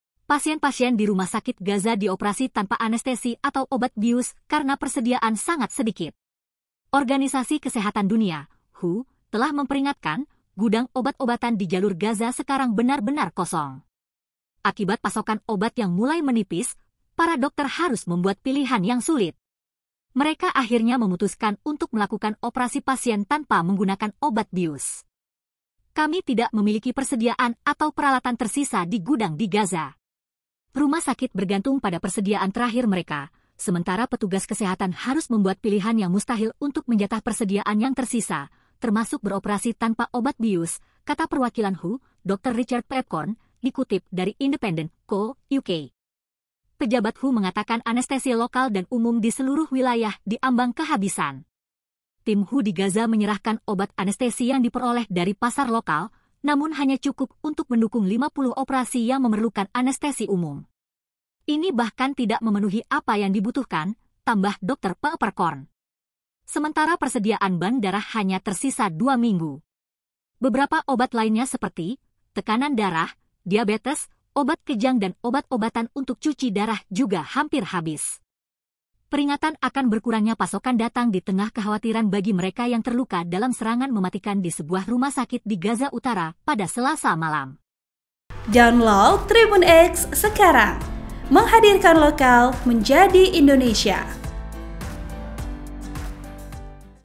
KONDISI HAMAS MIRIS? Persediaan Obat Menipis hingga Pasien di Gaza Dioperasi Tanpa Bius
TRIBUN-VIDEO.COM - Pasien-pasien di rumah sakit Gaza dioperasi tanpa anestesi atau obat bius karena persediaan sangat sedikit.

Organisasi Kesehatan Dunia (WHO) telah memperingatkan, gudang obat-obatan di jalur Gaza sekarang benar-benar kosong.

Akibat pasokan obat yang mulai menipis, para dokter harus membuat pilihan yang sulit.

Mereka akhirnya memutuskan untuk melakukan operasi pasien tanpa menggunakan obat bius.

Kami tidak memiliki persediaan atau peralatan tersisa di gudang di Gaza. Rumah sakit bergantung pada persediaan terakhir mereka, sementara petugas kesehatan harus membuat pilihan yang mustahil untuk menjatah persediaan yang tersisa, termasuk beroperasi tanpa obat bius," kata perwakilan WHO, Dr Richard Peepkorn, dikutip dari independent.co.uk.

Pejabat WHO mengatakan anestesi lokal dan umum di seluruh wilayah di ambang kehabisan.

Tim WHO di Gaza menyerahkan obat anestesi yang diperoleh dari pasar lokal, namun hanya cukup untuk mendukung 50 operasi yang memerlukan anestesi umum.

“Ini bahkan tidak memenuhi apa yang dibutuhkan,” tambah Dr Peeperkorn.

Sementara persediaan bank darah hanya tersisa dua minggu.

Beberapa obat lainnya seperti, tekanan darah, diabetes, obat kejang dan obat-obatan untuk cuci darah juga hampir habis.

Peringatan akan berkurangnya pasokan datang di tengah kekhawatiran bagi mereka yang terluka dalam serangan mematikan di sebuah rumah sakit di Gaza utara pada Selasa malam.

Operasi Dilakukan di Lapangan

Kementerian Kesehatan Gaza mengatakan petugas medis terpaksa melakukan operasi pada korban luka di lapangan.

"Tidak ada ruang operasi lagi karena banyaknya korban jiwa. Para dokter mencoba melakukan operasi di koridor rumah sakit tanpa anestesi apa pun," kata Dokter darurat di rumah sakit Al Shifa, Gaza, Mohamed Ghanem.

Dr Peeperkorn juga meminta untuk pasokan medis agar diperbolehkan masuk ke Gaza.

“Sistem kesehatan berada dalam kondisi lemah, dan keruntuhan akan segera terjadi kecuali bantuan diizinkan masuk. Saya memohon agar air, makanan, bahan bakar, dan pasokan medis diizinkan masuk ke Gaza. Waktu untuk membunyikan bel alarm telah habis. Gaza sedang runtuh tepat di depan mata kita," jelasnya.

Dia mengatakan bahwa truk WHO yang berisi perbekalan telah menunggu di perbatasan Mesir dengan Gaza selama lima hari.

Namun hingga saat ini, truk tersebut tidak diperbolehkan masuk.

"Tetapi belum dapat menyeberang, karena tidak ada koridor bantuan antara Mesir dan Israel yang disepakati,' tambahnya.

Sebagai informasi. serangan menghantam rumah sakit al-Ahli di Gaza pada Selasa malam.

Saat itu, rumah sakit tersebut telah menampung ratusan pengungsi.

Banyak korban dilarikan ke rumah sakit utama di Gaza yaitu al-Shifa.

(Tribunnews.com/Farrah Putri)